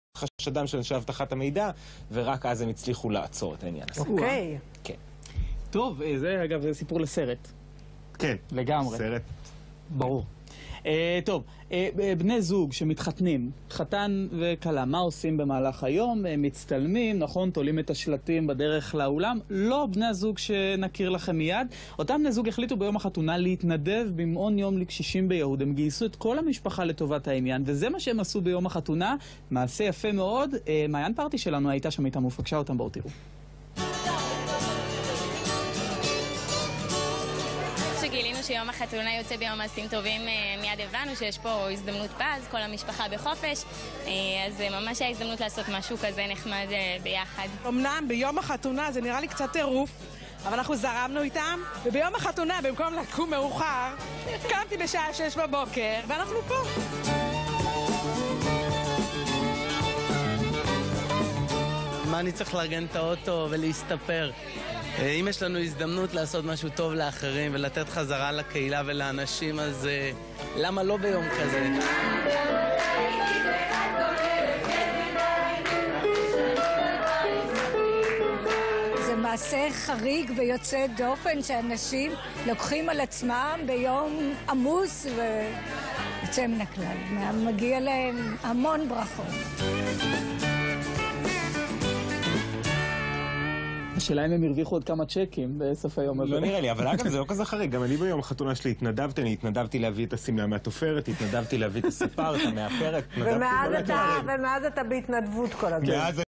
Tying the knot and volunteering on Good Deeds Day
The bride and groom invited their entire family to do good deeds at a senior center on Good Deeds Day 2016, and marked their special day by giving back to the community.